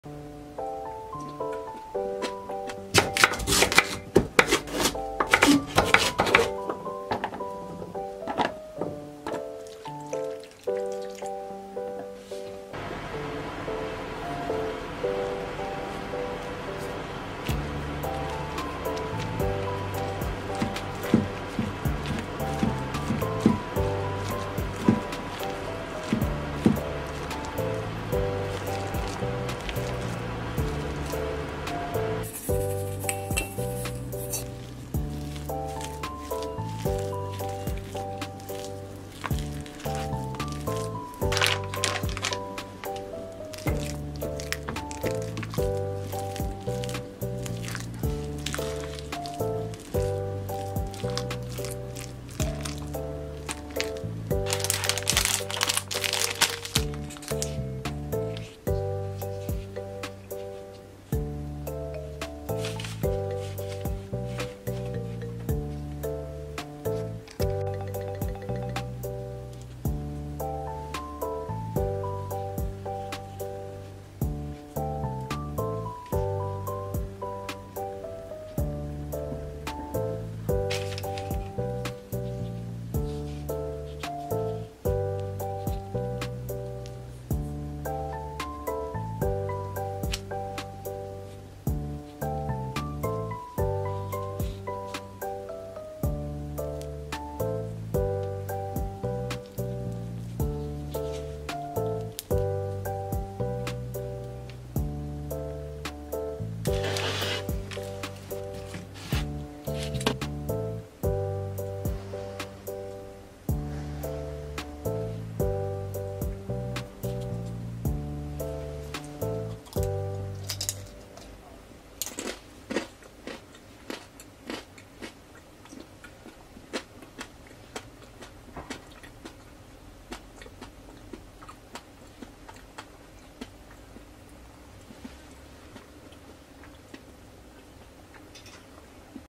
おばけスイートポテトの作り方
餃子の皮をおばけに見立ててみました